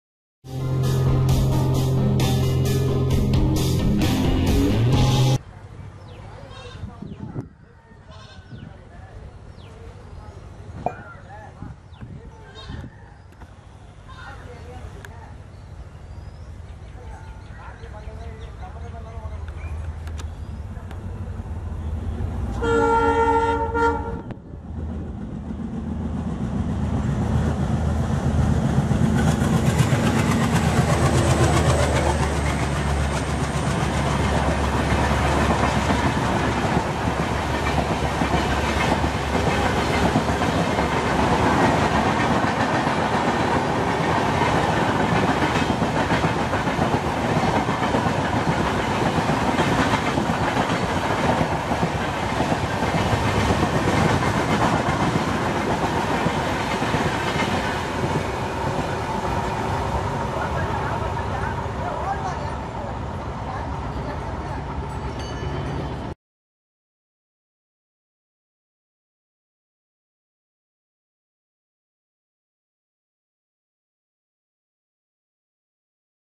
Volcanic Eruption by Cholan express | GOC-WDM-3A
Cholan express which was arriving from tirchy and bound to mayiladuthurai junction  gave an heavy smoke by Green Alco WDM-3A near kutralam station